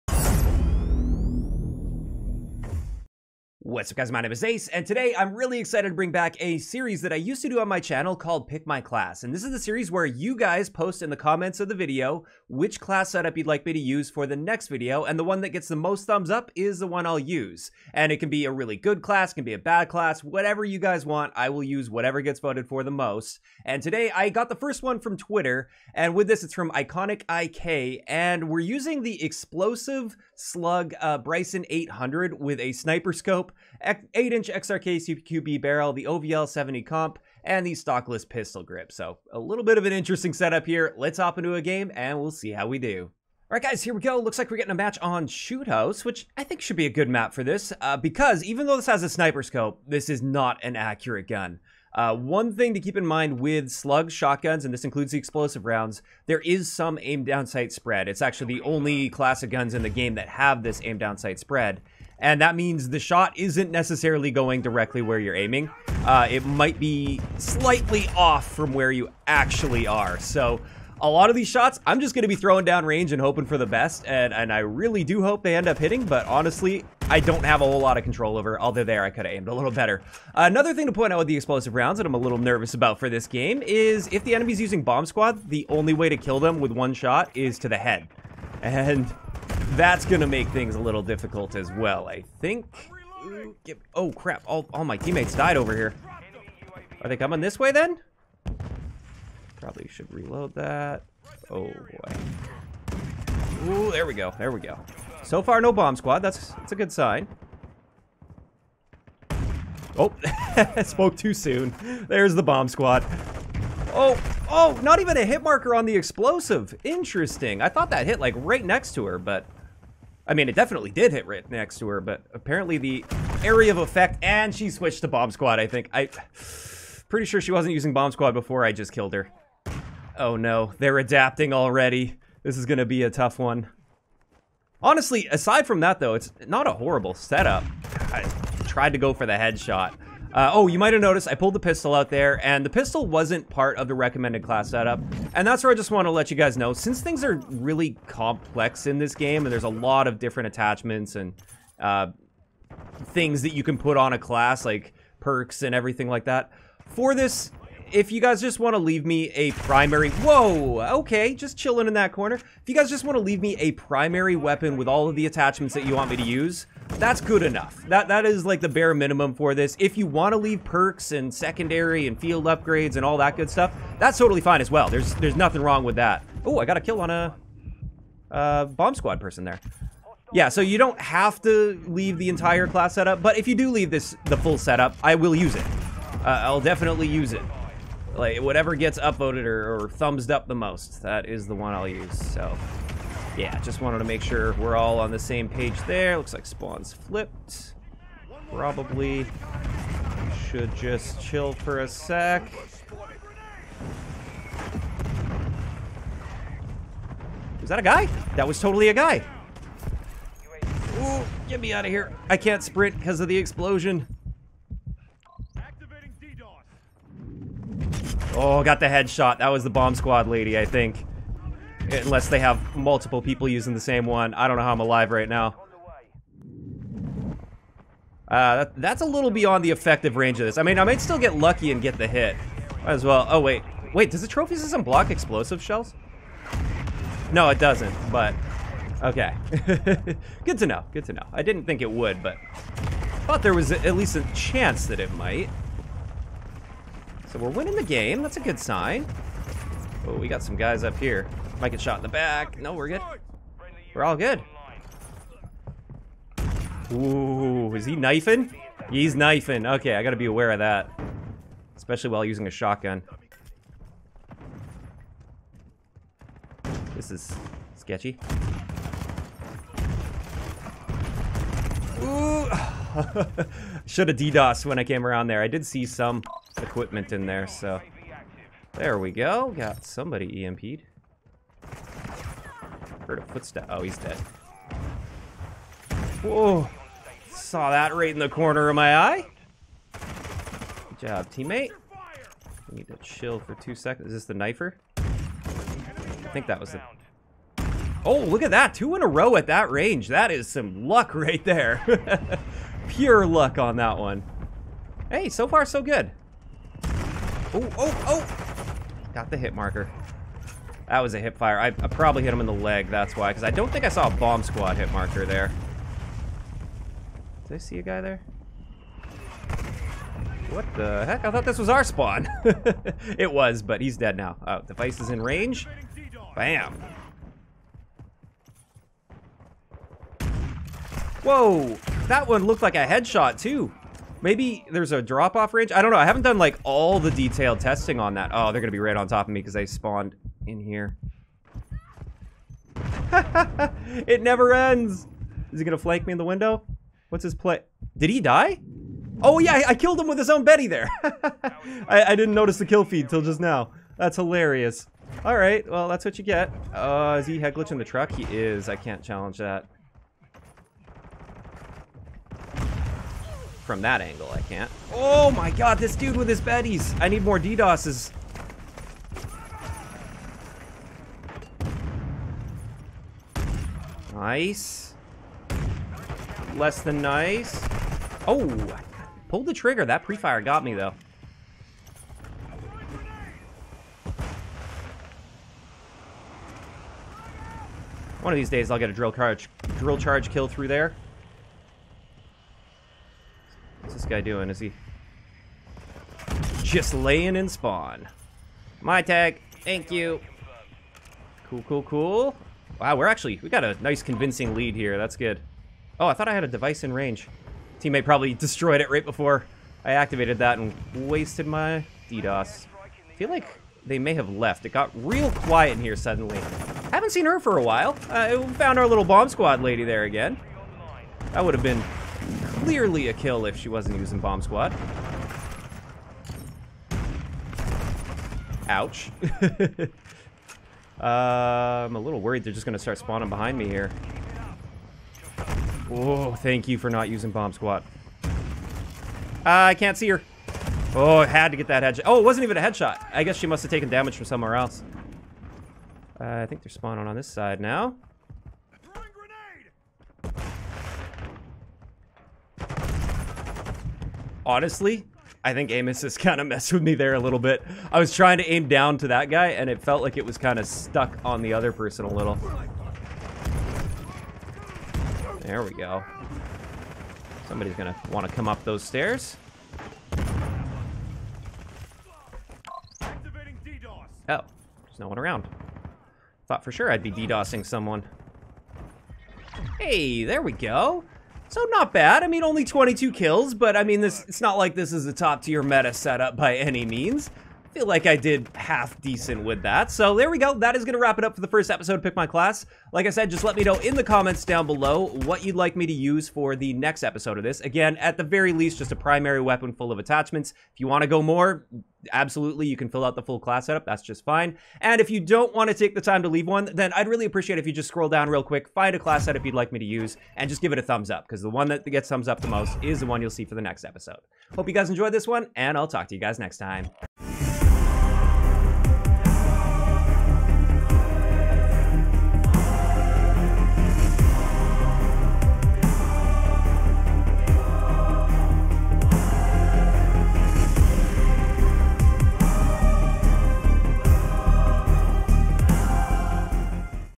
Why Explosive Slugs Suck on Shotguns... | Pick My Class!
Welcome to Pick my Class! This is the series where I use whatever setup gets the most thumbs up in the comments of the most recent episode so leave a setup in the comments and a thumbs up on any classes you'd like to see me use next!

0:00 Intro/Setup
0:43 Gameplay